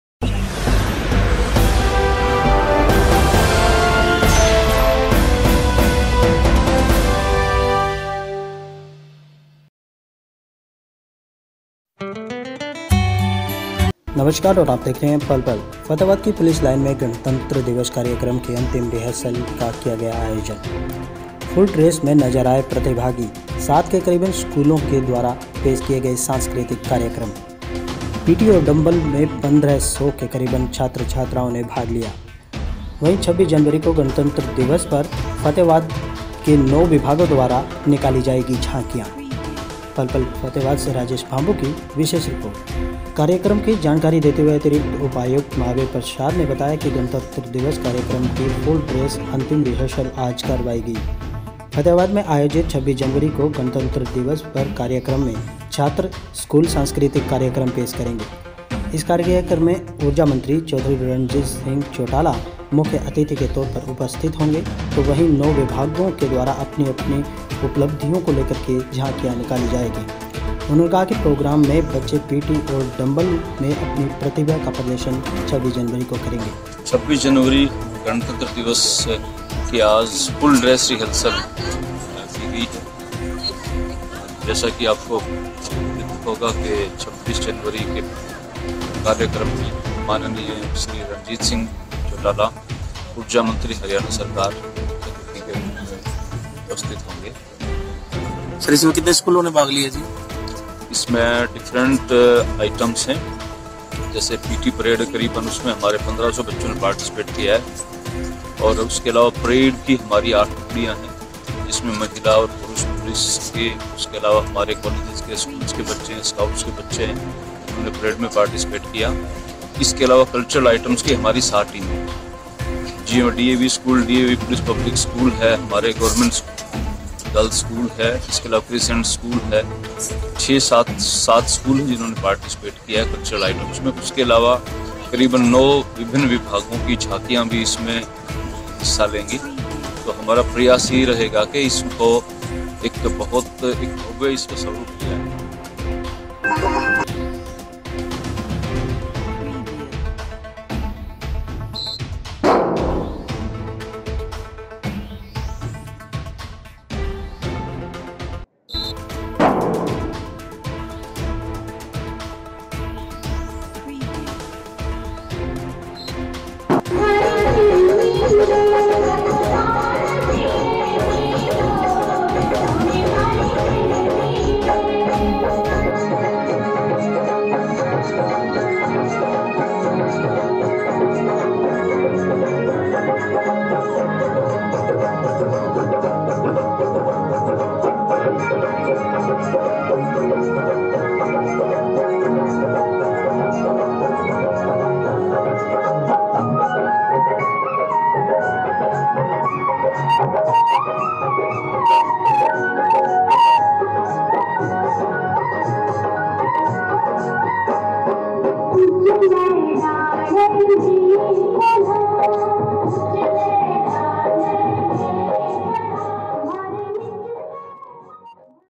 गणतंत्र दिवस कार्यक्रम की अंतिम रिहर्सल का किया गया आयोजन
फतेहाबाद की पुलिस लाइन में आज गणतंत्र दिवस कार्यक्रम की अंतिम रिहर्सल का आयोजन किया गया। इस रिहर्सल कार्यक्रम में मुख्य अतिथि के तौर पर फतेहाबाद के अतिरिक्त उपायुक्त महाबीर प्रसाद उपस्थित रहे। इस कार्यक्रम में 7 के करीब स्कूलों ने अपने सांस्कृतिक कार्यक्रमो को लेकर अंतिम रिहर्सल की। वहीं 1500 बच्चों ने पीटी और डंबल मे अपना जौहर भी दिखाया। कार्यक्रम की जानकारी देते हुए अतिरिक्त उपायुक्त महाबीर प्रसाद ने बताया कि गणतंत्र दिवस कार्यक्रम की फुल ड्रेस अंतिम रिहर्सल आज करवाई गई है। फतेहाबाद में आयोजित 26 जनवरी को गणतंत्र दिवस कार्यक्रम में छात्र स्कूल सांस्कृतिक कार्यक्रम पेश करेंगे। इस कार्यक्रम में ऊर्जा मंत्री रणजीत चौटाला मुख्य अतिथि के तौर पर उपस्थित रहेंगे। वहीं 9 विभागों के द्वारा अपनी उपलब्धियों को लेकर झांकियां निकाली जाएंगी।
उन्होंने कहा कि प्रोग्राम बच्चे पीटी और डंबल में अपनी प्रतिभा का प्रदर्शन 26 जनवरी को करेंगे। आज आयोजित की गई अंतिम फुल ड्रेस रिहर्सल में जो कमियां थी उसे दूर करने का प्रयास किया गया है।